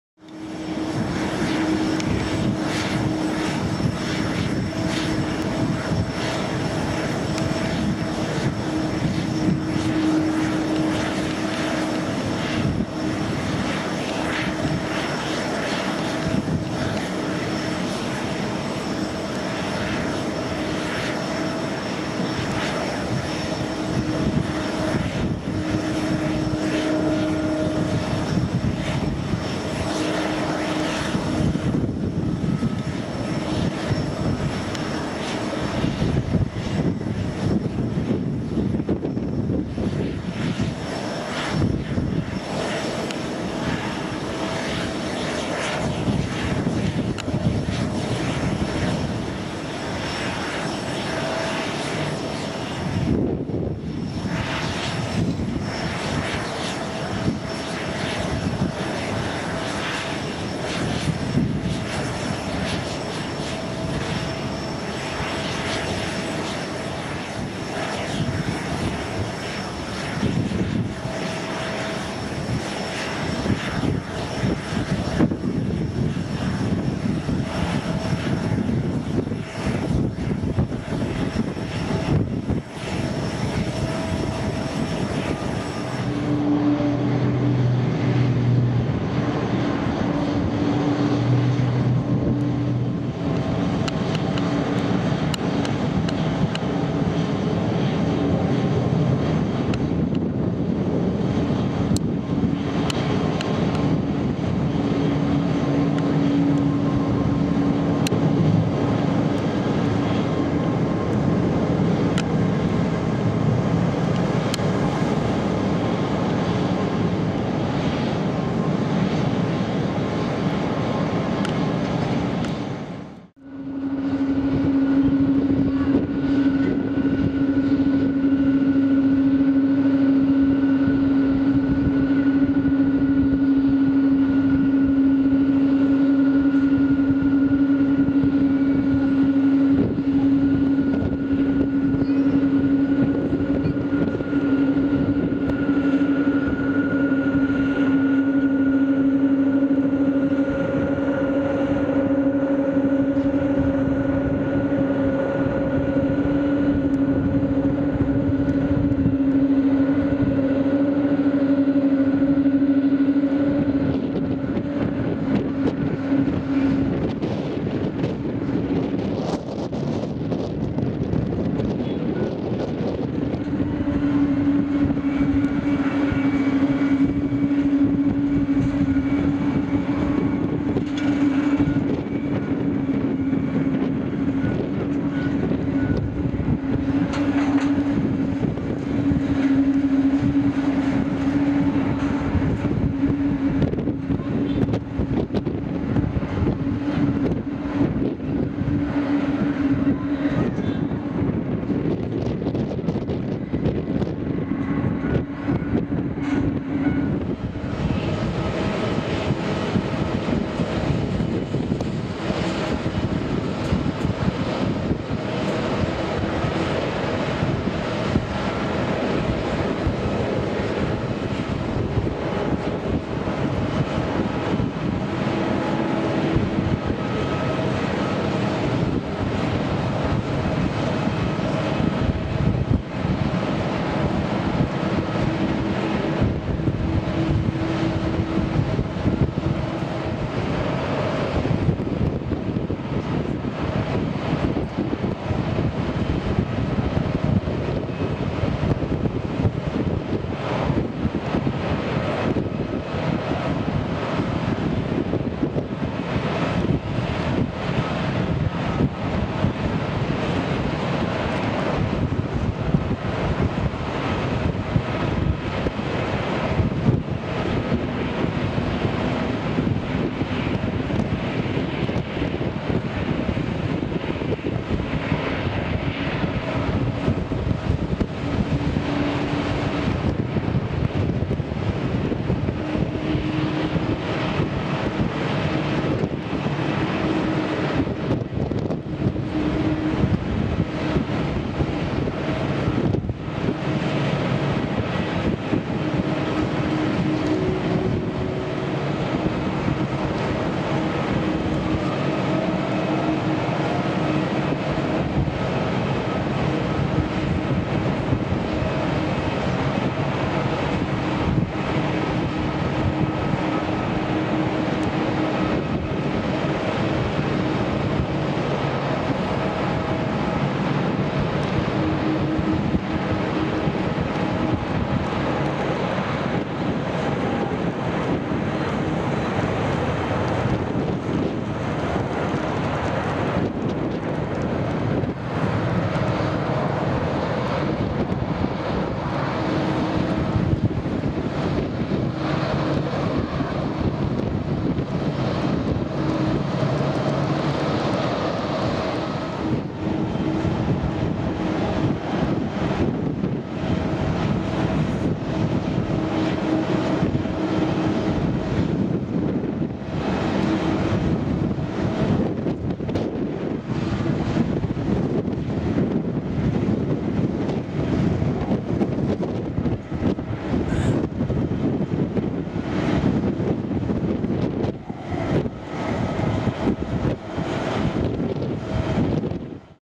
ユナイテッド航空 in 羽田！United Airlines B777-200ER and B787-9 - Tokyo International Airport 2017
OPR: United Airlines (UAL/UA), USA
SCN: Taxiing, takeoff and landing
LOC: Tokyo International Airport (RJTT/HND), Japan
DAT: March 3rd and 11th, 2017